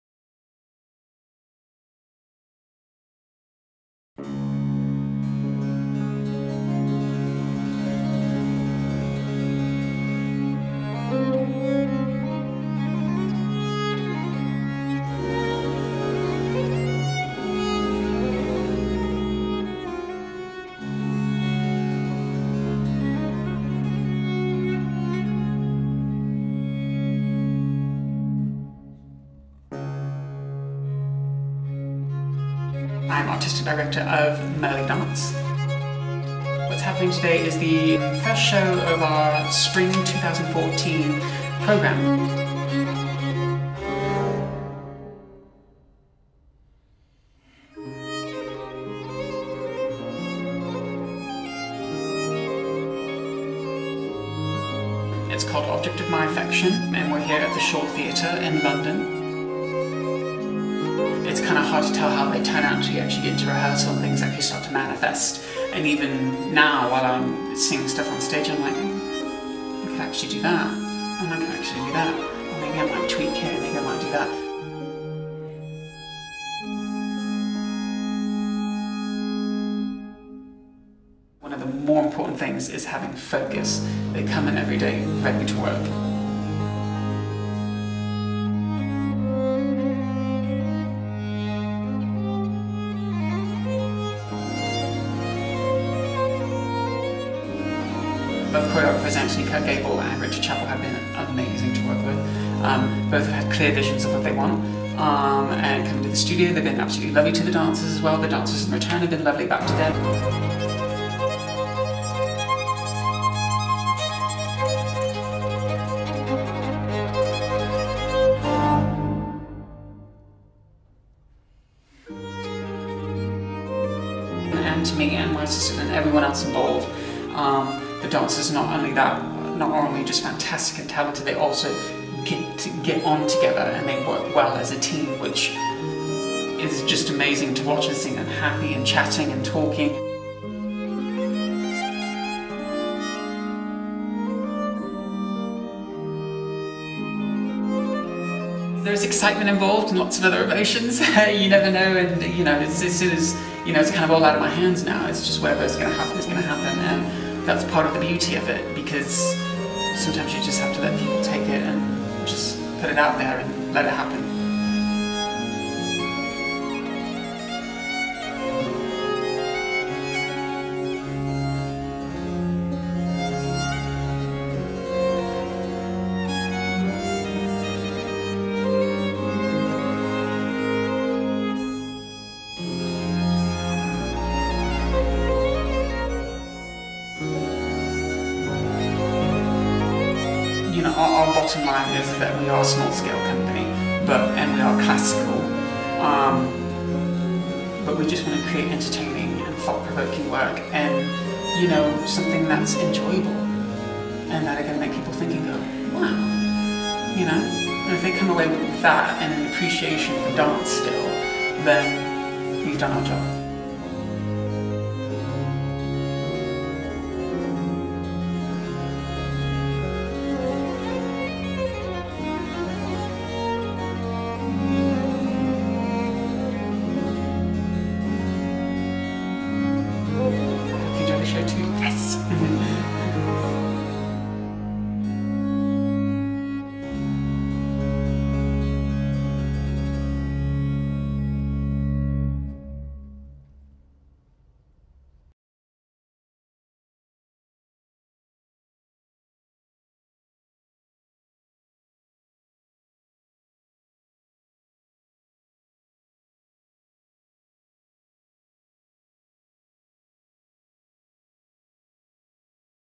MurleyDance 2014 Spring Programme
A short film about our Spring 2014 programme, 'Object Of My Affection', which was filmed over a 5 week period during rehearsals and during the premiere in London on the 27th April 2014.  We hope you enjoy!